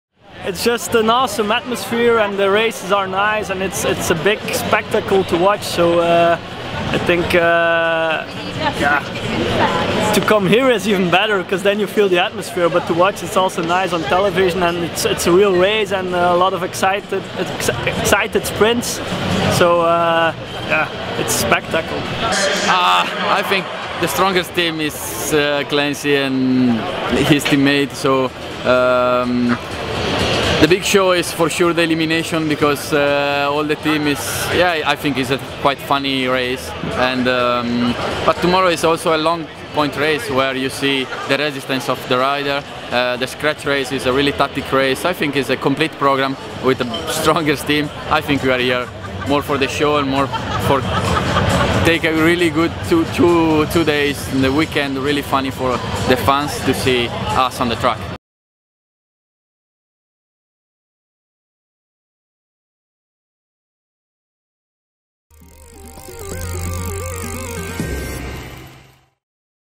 Fast and furious action from Revolution Cycling Champions League
It's day two in Manchester, where JLT Condor began the day in the lead with Team Pedalsure and Maloja Pushbikers second and third respectively with Cannondale-Drapac the highest placed WorldTour team in fourth. The action is live on Eurosport on Saturday, details and more event news